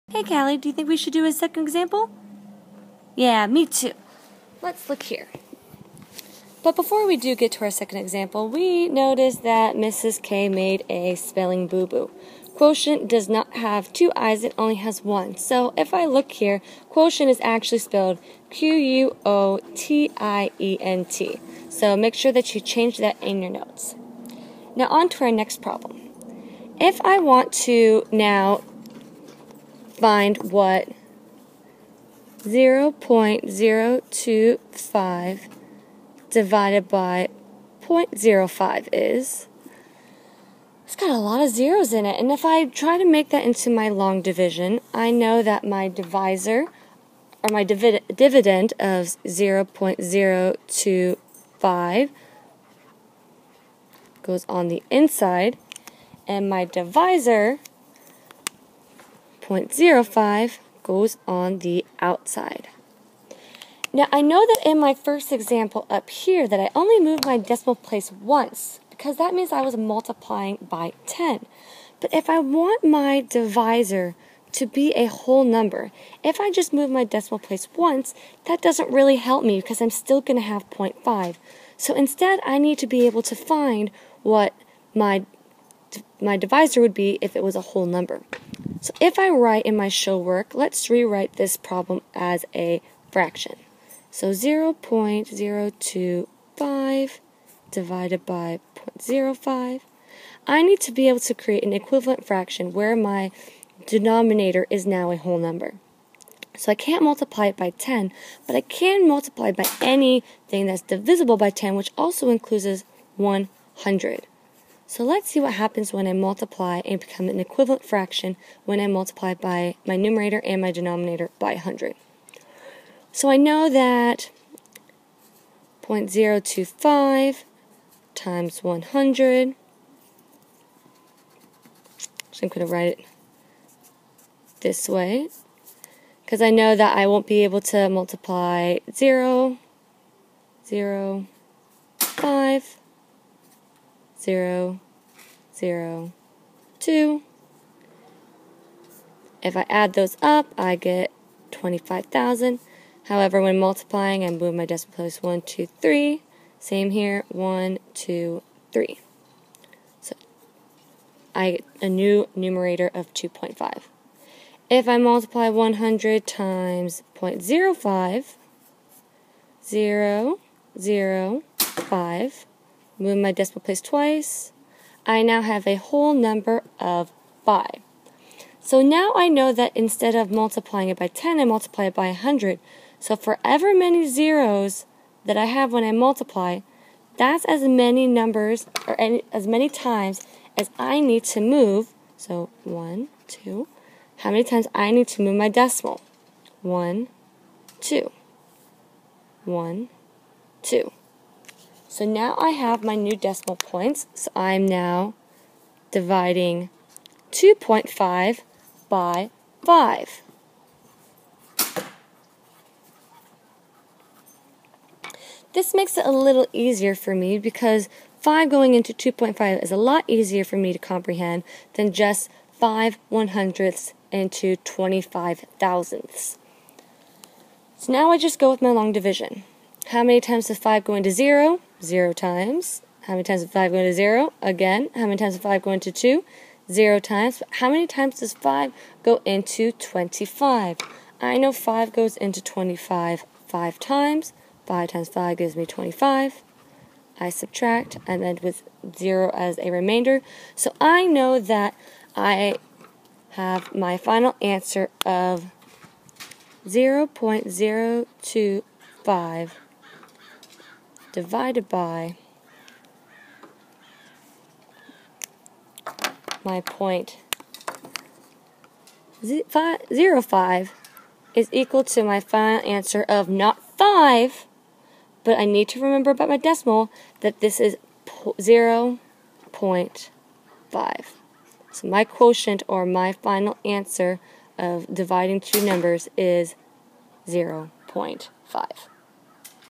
Dividing Decimals - Example #2
See how to divide decimals with four examples and some take away rules. See that decimals ARE COOL!